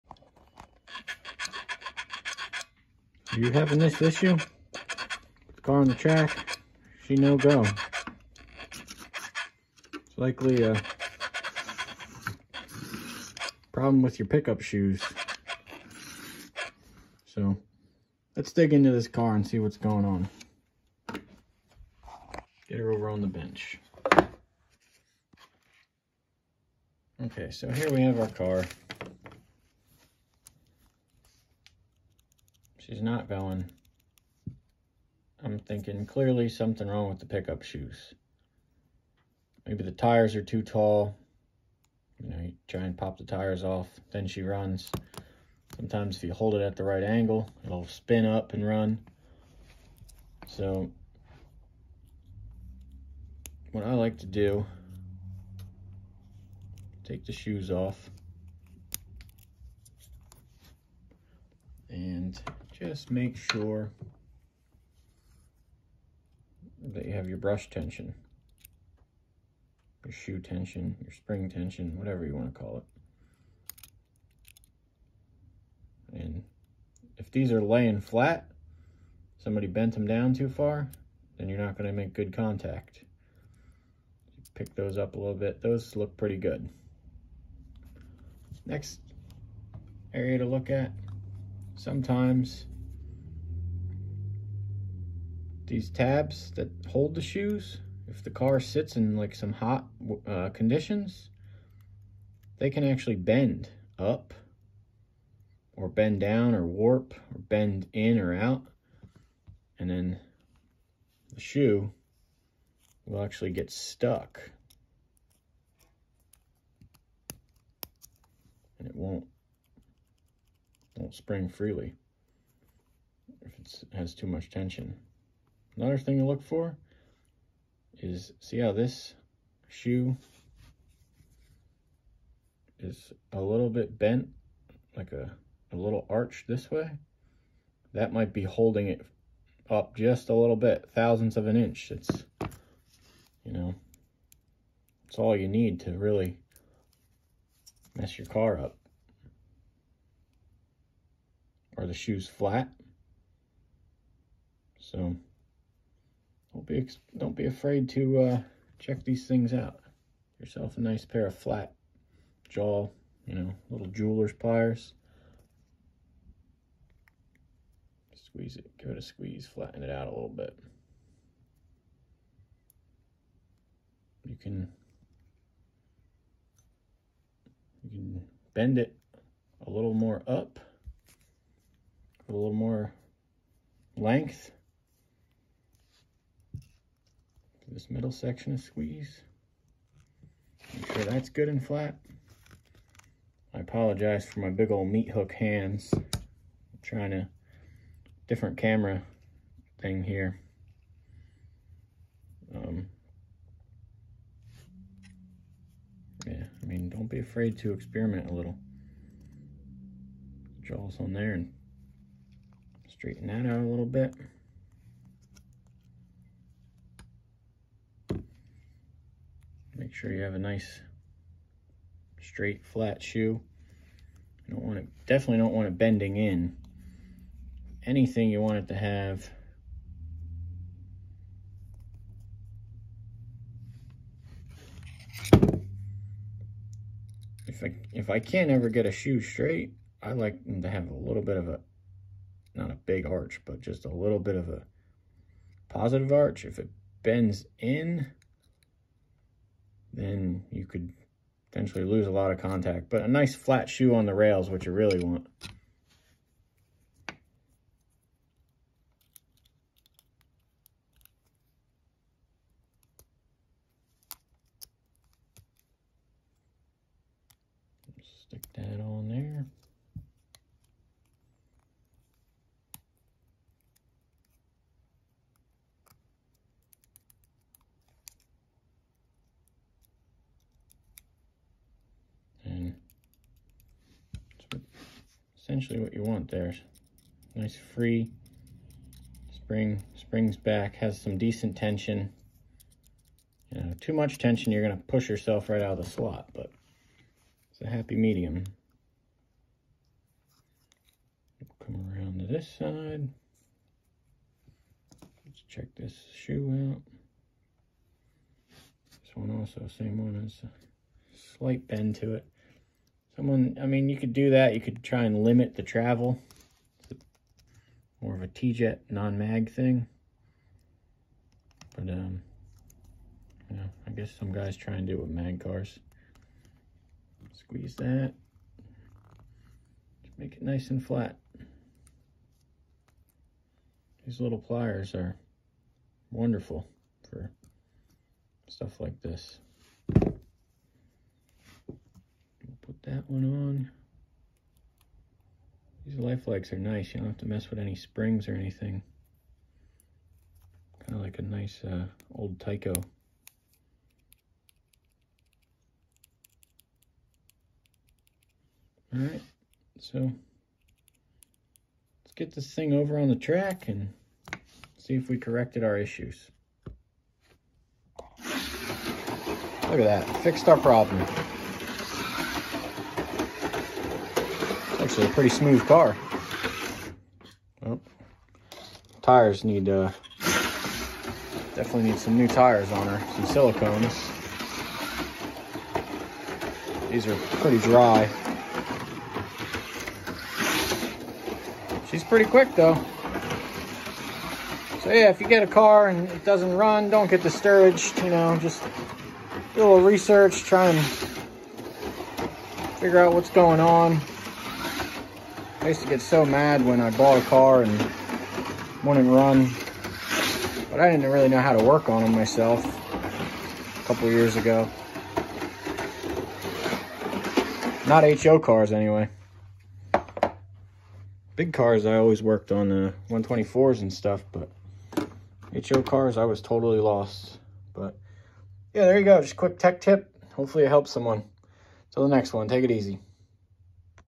Tech tips for Life Like Slot cars
Today we’re going to the workbench to share a quick tech tip. This trick will work for many brands of slot car but is a more common problem with theses life likes. Good shoe contact is key to a smooth running slot car.